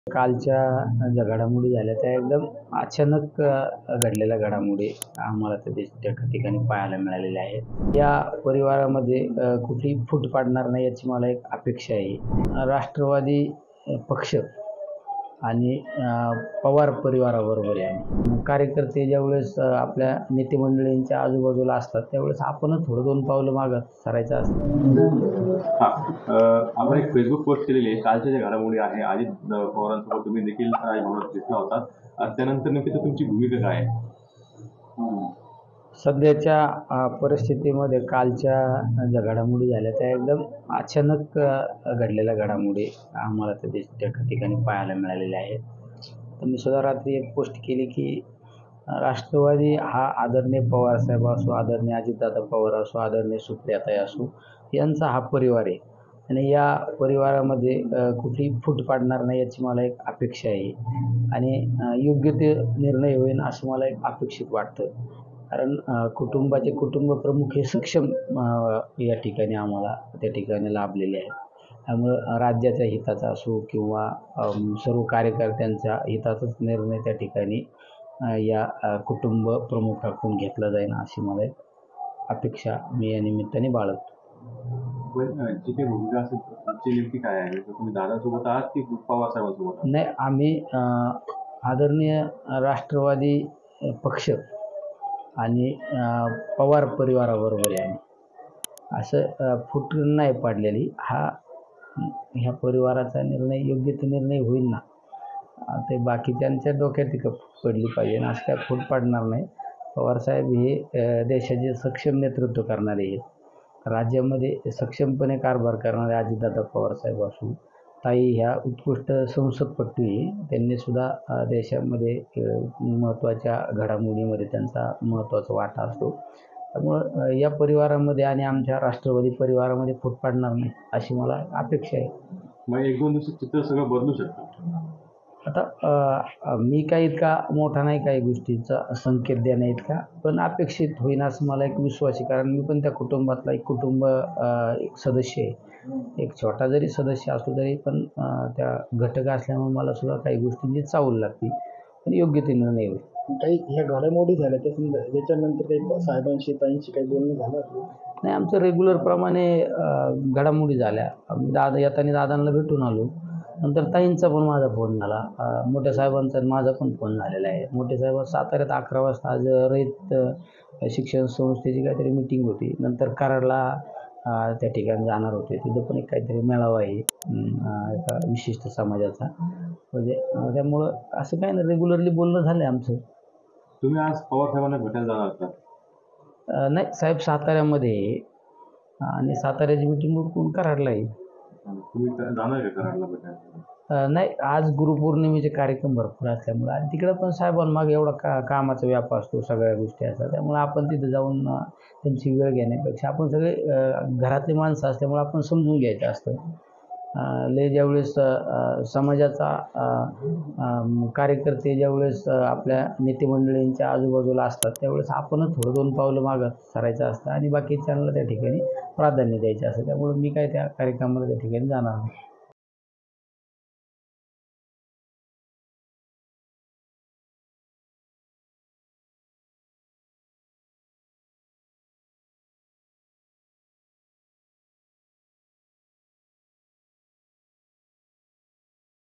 Nilesh Lanke on NCP Crisis | राष्ट्रवादी परिवारात फूट पडणार नाही, दोन दिवसात... | LetsUpp Marathi
राष्ट्रवादी कॉंग्रेसचे नेते अजित पवार यांनी उपमुख्यमंत्रिपदाची शपथ घेतल्यानंतर राज्यातील राजकारणात एकच खळबळ उडाली आहे. त्यात आता राष्ट्रवादीचे आमदार निलेश लंके नेमके कोणासोबत राहणार आहेत? असं विचारल्यावर त्यांनी थेट सांगून टाकलं.